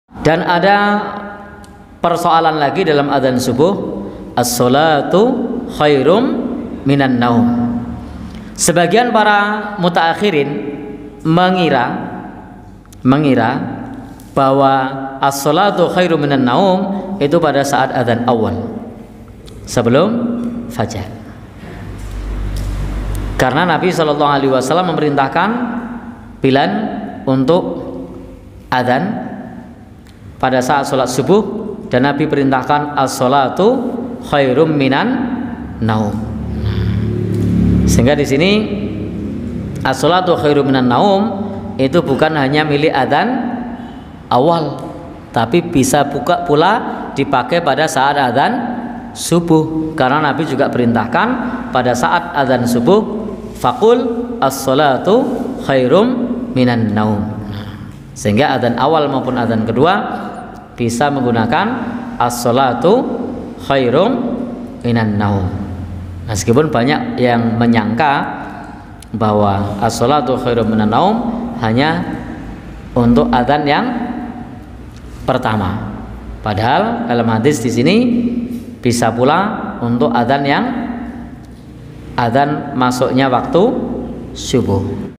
Persoalan ashsholatu khoirum minan naum pada saat adzan subuh | ustadz mujiman
Persoalan ashsholatu khoirum minan naum

ustadz mujiman

Apakah adzan subuh pakai ashsholatu khoirum minan naum?